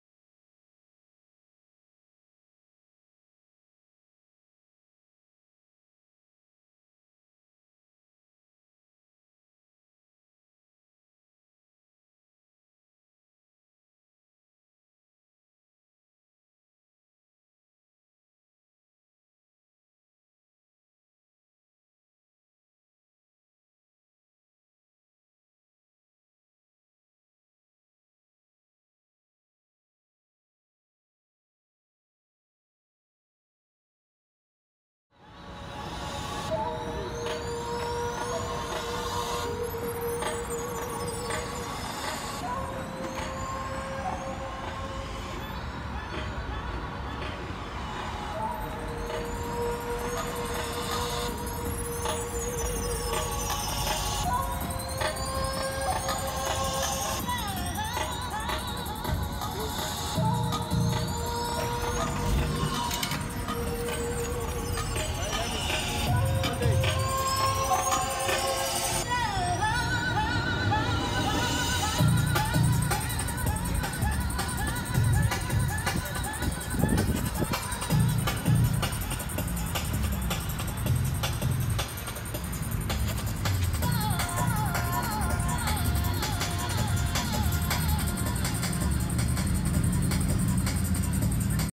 Nicolas Cage 1994 Porsche 911 Turbo from Bad Boys movie, Ceramic Coating installation on paint
Extremely rare 911, fastest car in 90-s, only 7000 miles and used as a weekend fun car by new owner, Ceramic Protection and also extreme gloss shine.